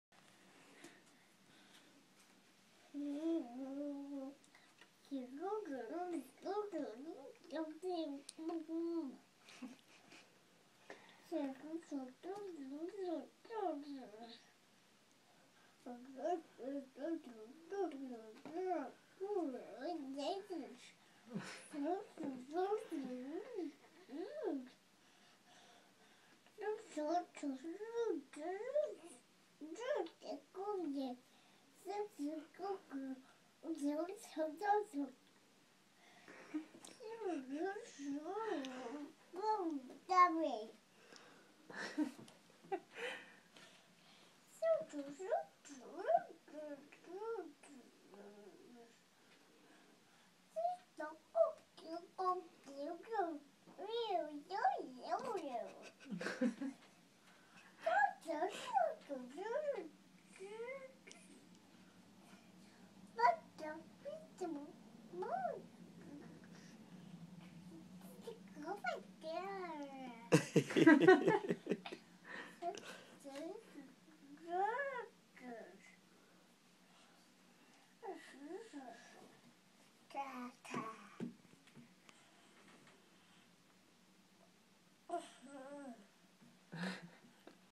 Blakely Reads a Book
Blakely reads us Miss Spider's Tea Party